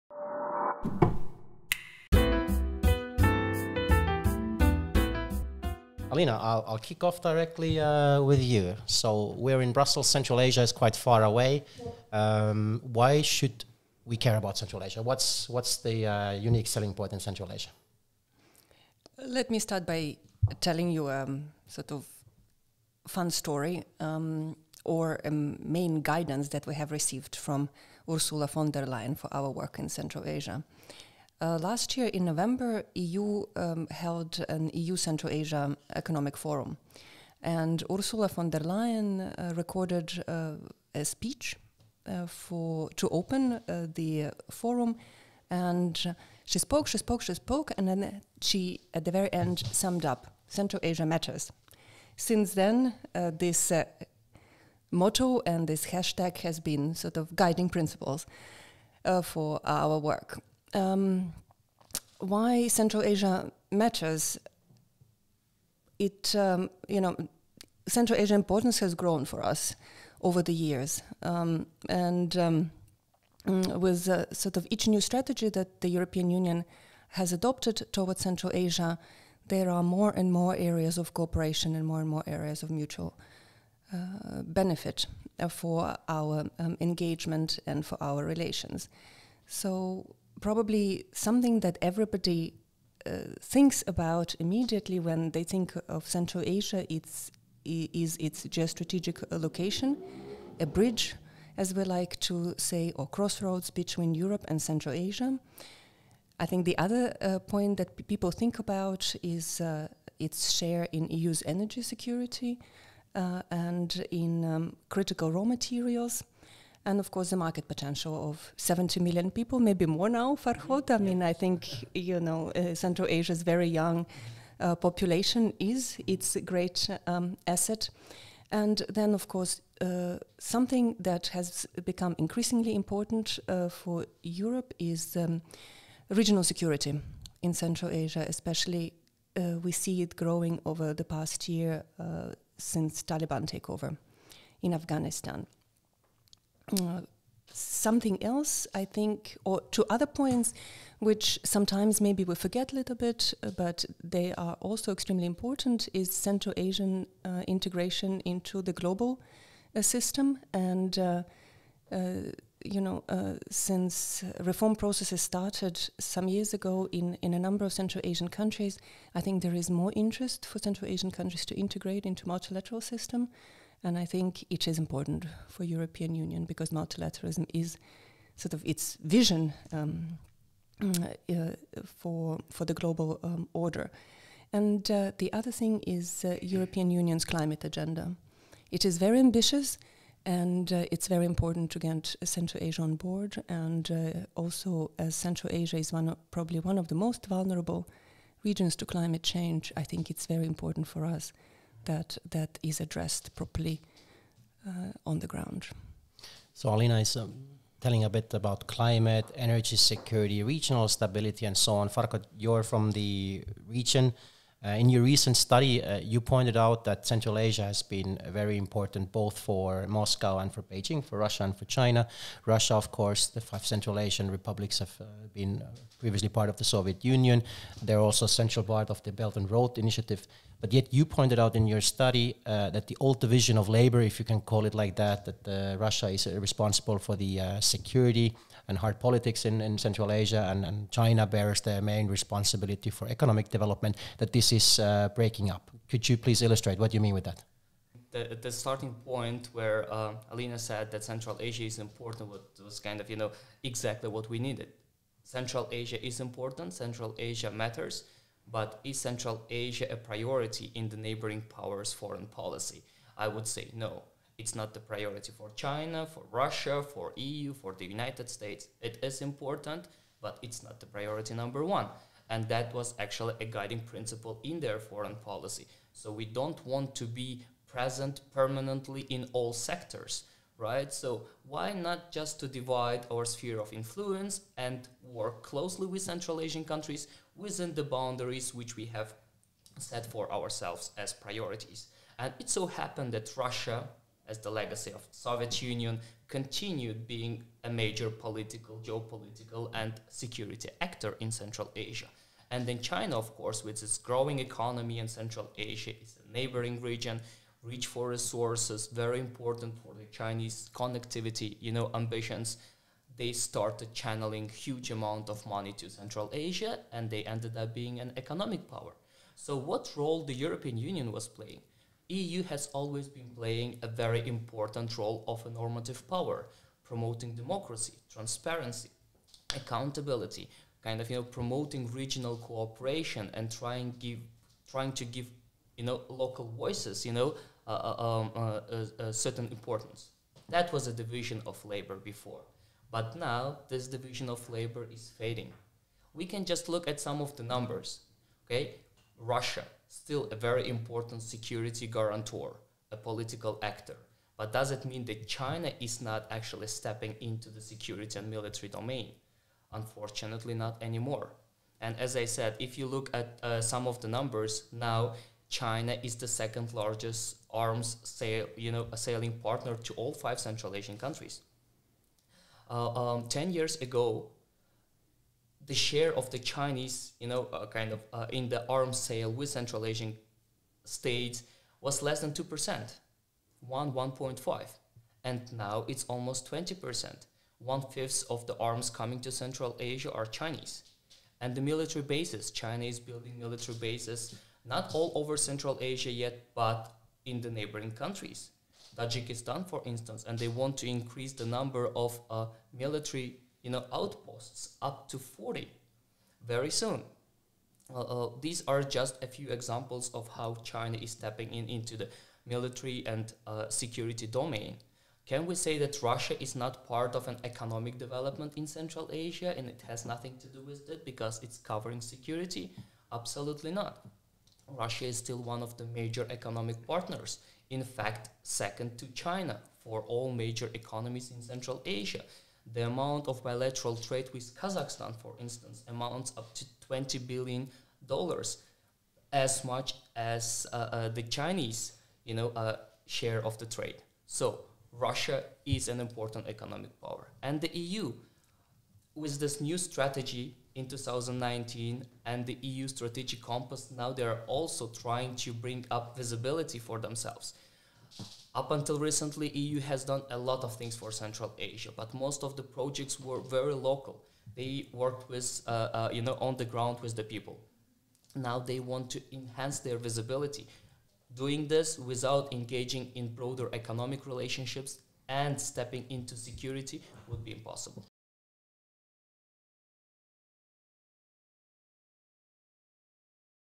Why Central Asia matters to Europe (extract 1)
“Central Asia matters” was European Commission President Ursula von der Leyen’s main message during the 2021 economic forum in Central Asia. The five Central Asian countries are located between China, Russia and Europe and boast rich natural resources. In this video we take a closer look at the region and discuss why it´s gaining importance for European policy makers.